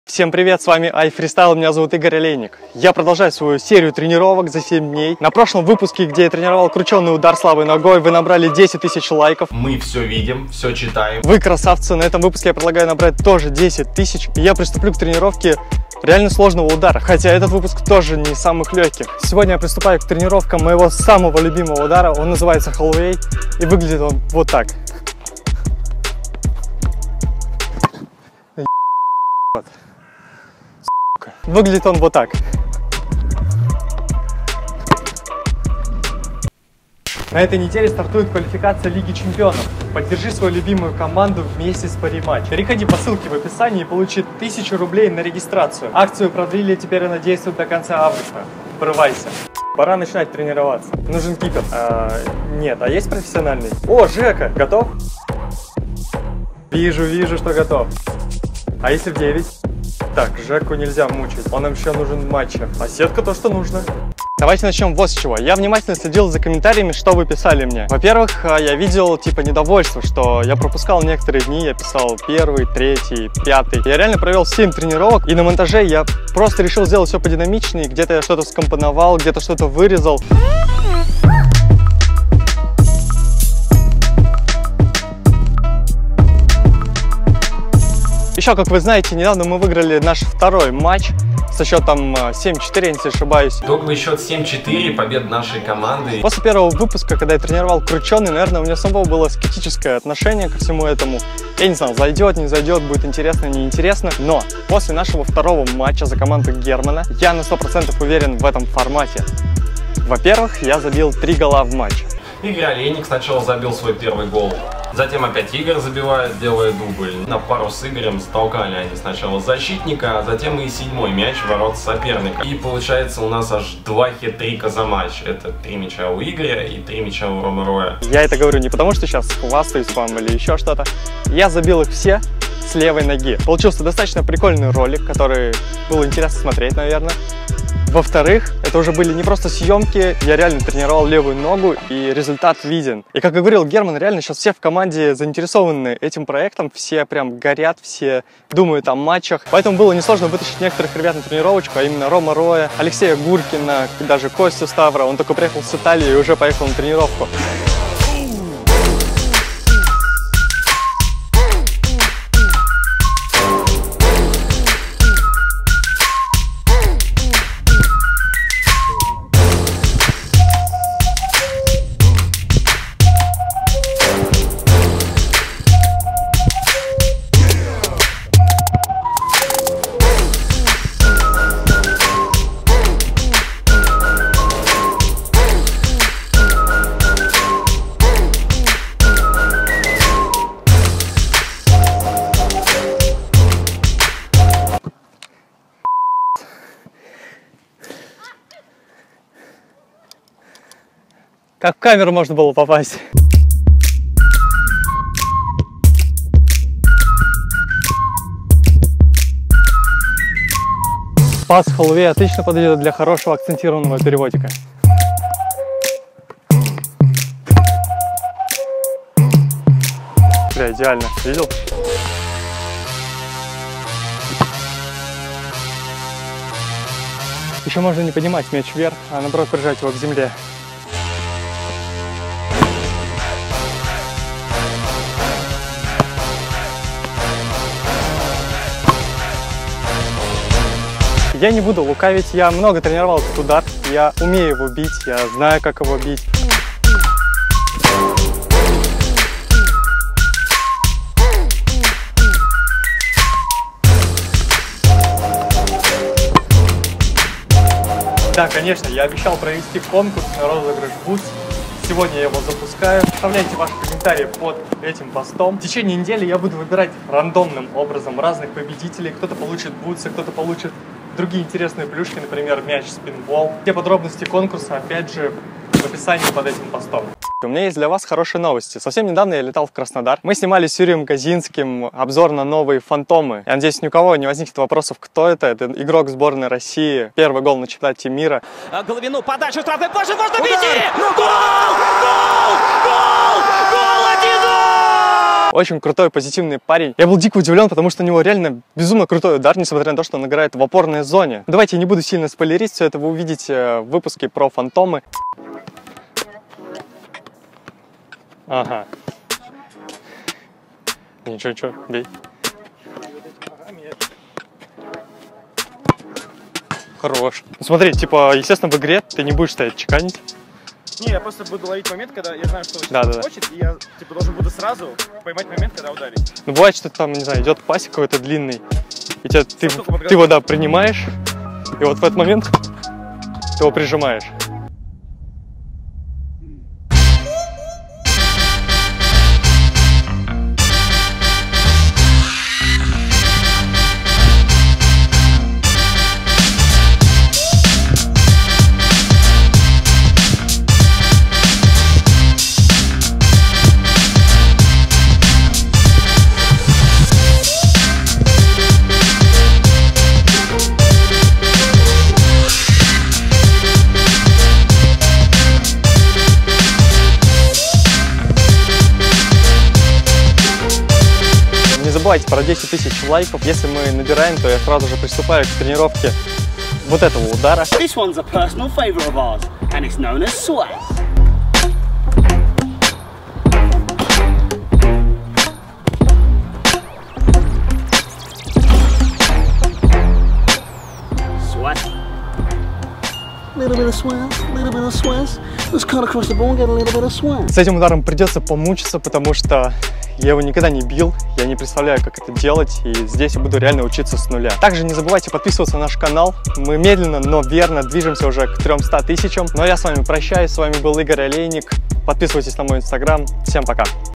Удар THE H-V ЗА 7 ДНЕЙ?! Удар с отскока /// Тренировка с Рома Рой, STAVR (СТАВР) и Леша Гуркин
Продолжаем серию тренировок ударов за 7 дней. В этом выпуске я выбрал свой любимый удар - THE H-V ЗА 7 ДНЕЙ. Со мной потренировались Рома Рой, STAVR (СТАВР) и Леша Гуркин. Думаю, мы готовы к следующему матчу в команде Германа